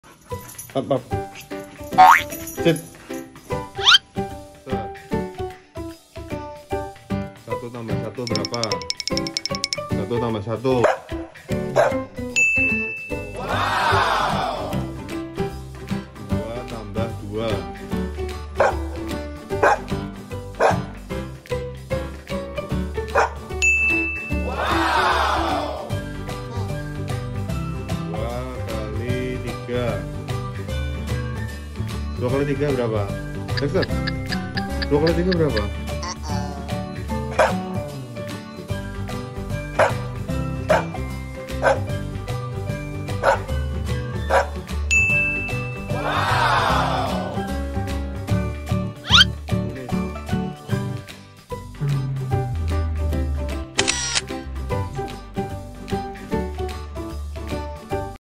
Pitbull Galak Pintar Berhitung 🥰
Anjing galak, itulah persepsi awal terhadap pitbull. Untuk itu ajak selalu pitbull anjing kesayangan anda untuk bermain, supaya selalu menjadi anjing patuh, anjing ceria dan anjing manja. Ajarkan selalu pola dasar kepatuhan supaya selalu menjadi anjing lucu dan anjing penurut meskipun bertampang anjing ganas. Sayangi setiap saat supaya selalu menjadikan pitbull anjing setia 🥰

Pitbull juga bisa diajarkan untuk menjadi anjing yang pinter berhitung. Latih setiap hari maka pitbull anda akan menjadi anjing pandai berhitung